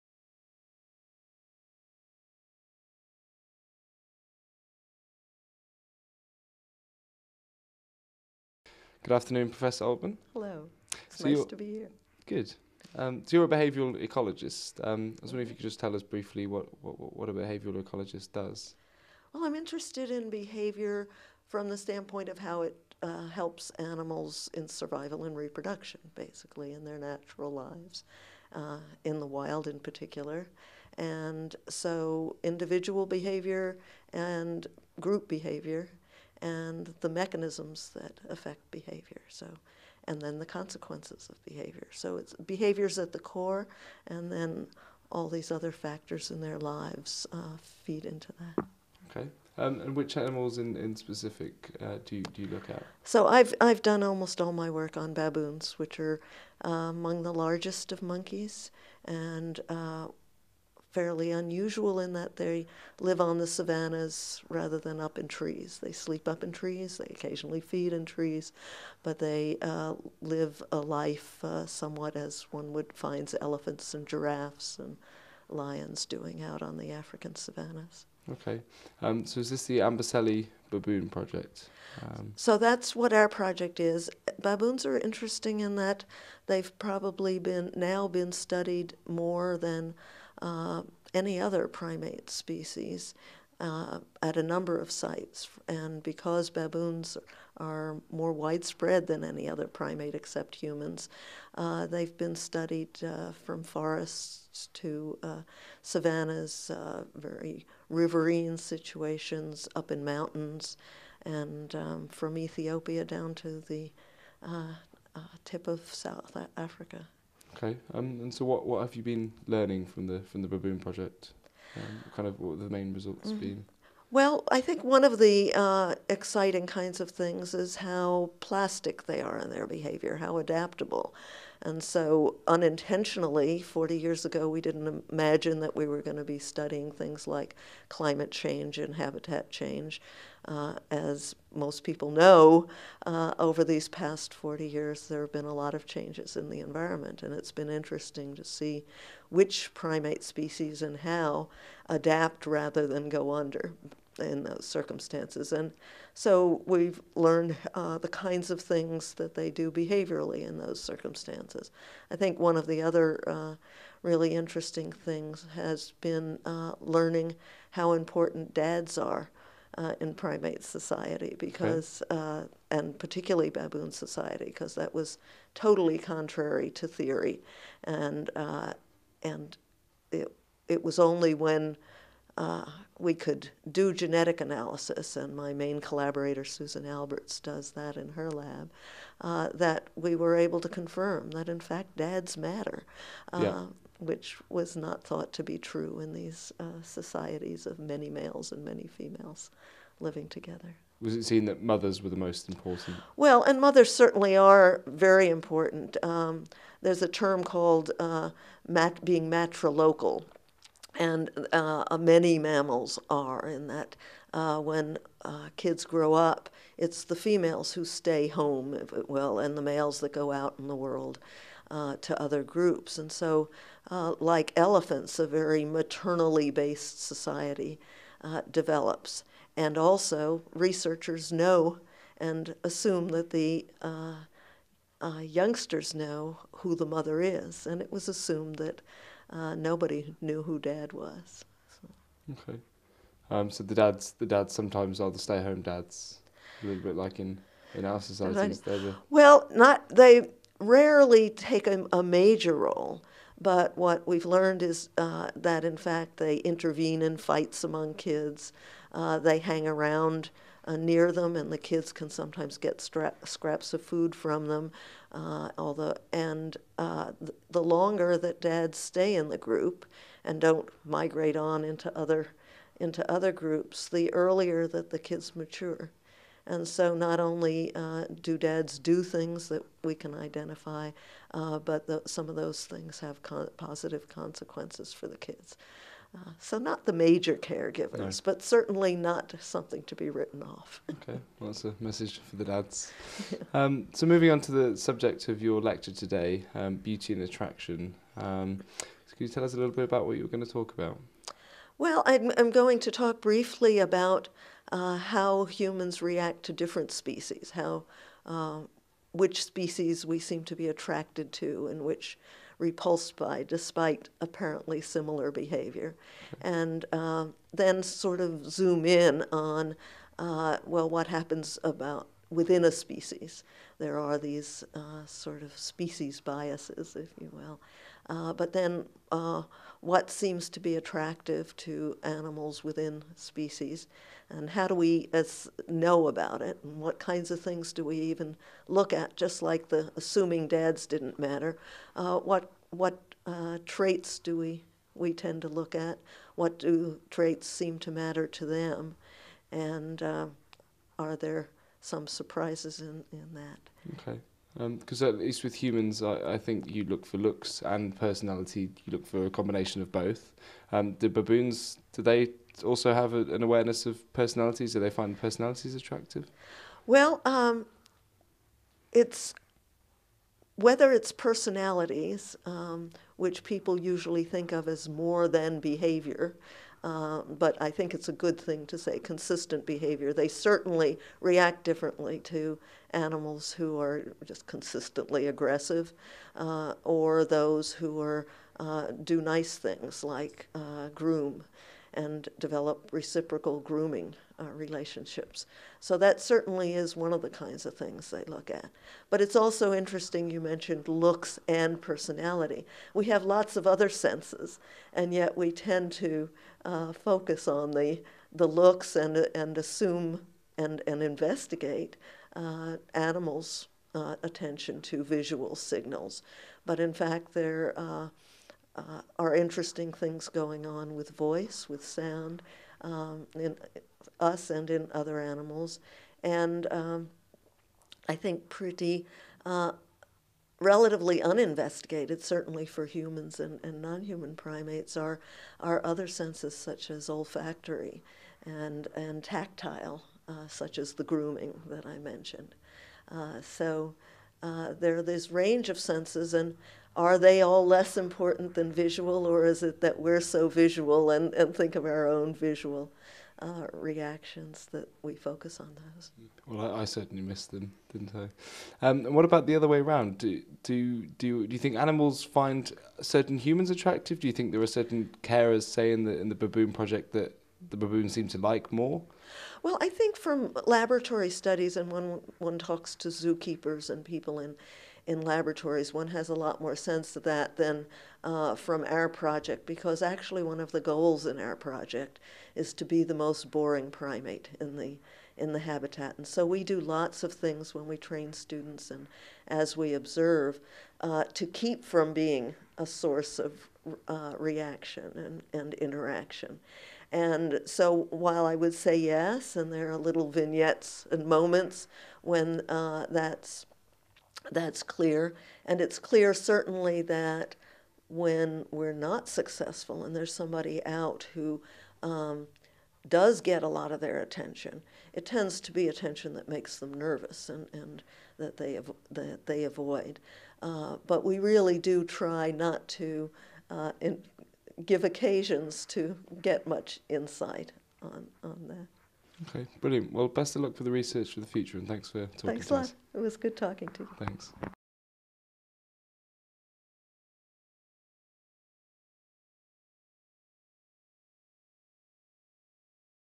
Darwin Lecture Series 2011: Professor Jeanne Altmann
James Cox interviews Professor Jeanne Altmann from Princeton about her work studying the behaviour of baboons.

Professor Altmann visited the University of Cambridge on the 18th of February 2011 to give her talk, "Beauty and Attraction: in the eye of the beholder" as part of the 2011 Darwin College Lecture Series. A video of her talk, as well as all the other in the series may be found at:

as can CU-TV interviews with the other speakers.